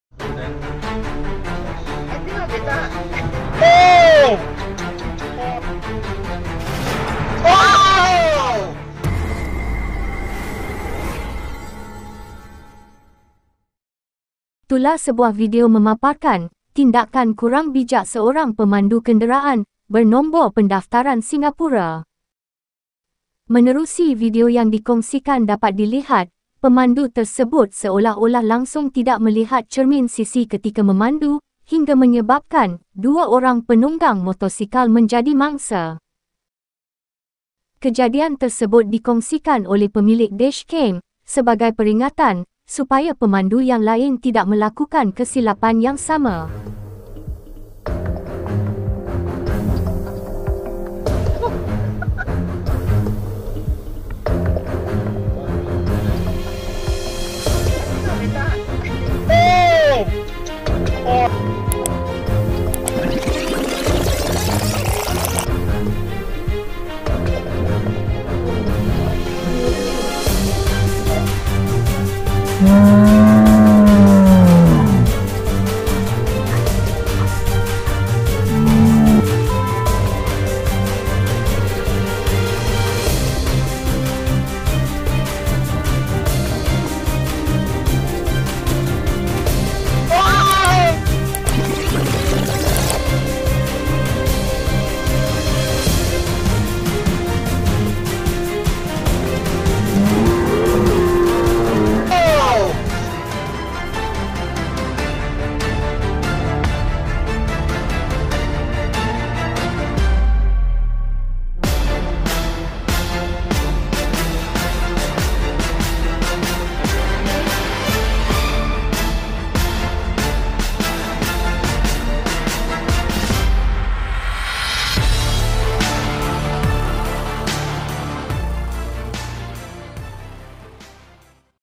DUA KALI DIA NI BUAT HAL. TAK RETI GUNA CERMIN SISI KE
DUA KALI DIA NI BUAT HAL. TAK RETI GUNA CERMIN SISI KE memaparkan tindakan seorang pemandu yang dilihat seakan-akan tidak menggunakan cermin sisi hingga menyebabkan dua orang penunggang motosikal menjadi mangsa.